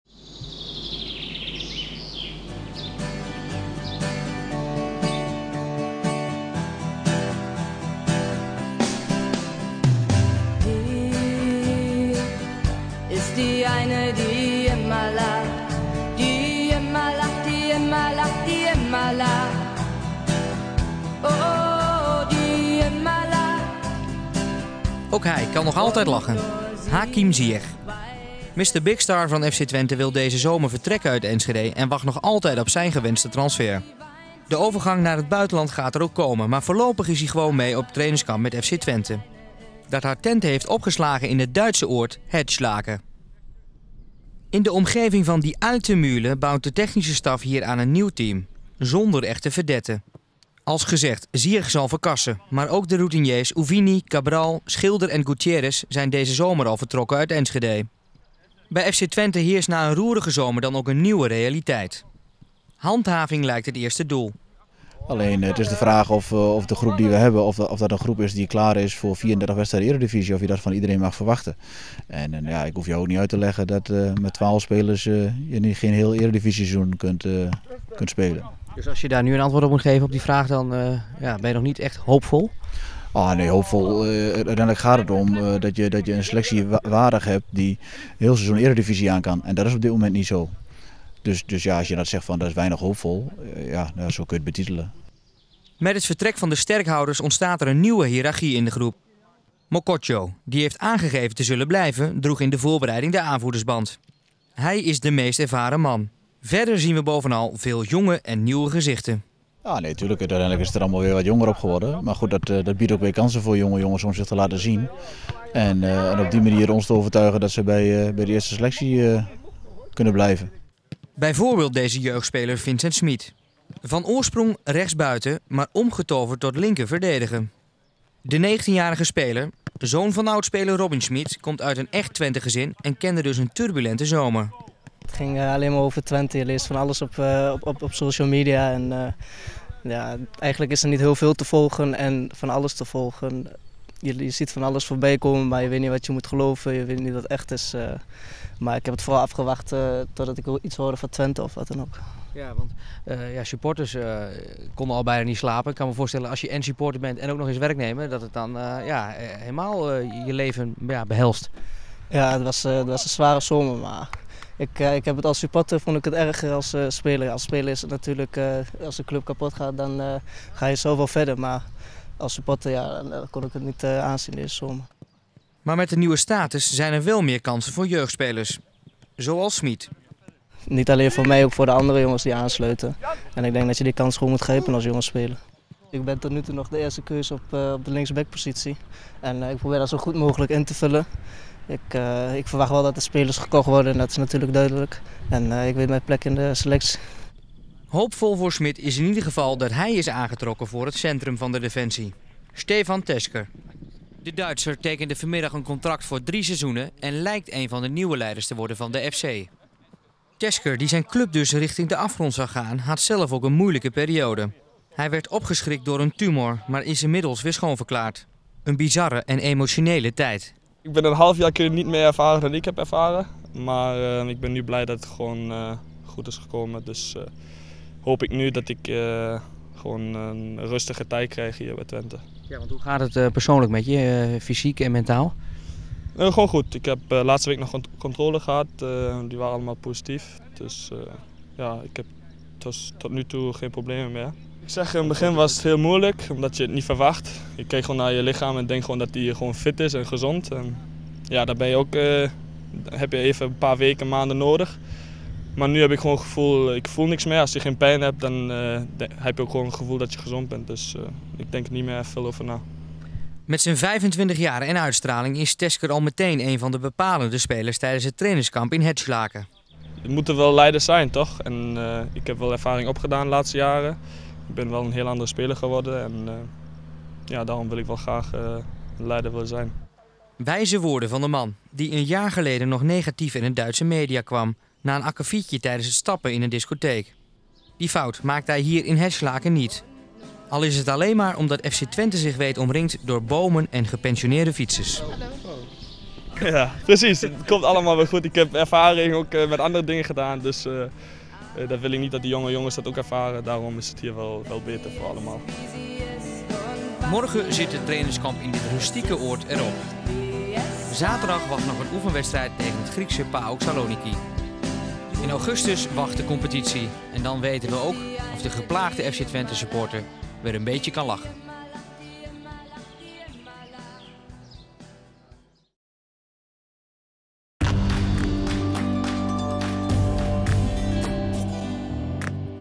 Stefan Thesker tekent voor drie seizoenen bij FC Twente
De komst van Stefan Thesker naar FC Twente is definitief. De 25-jarige Duitse verdediger, die het afgelopen half jaar op huurbasis overkwam van Greuther Frth, maakt nu definitief de overstap naar De Grolsch Veste. Thesker tekent een contract voor drie seizoenen.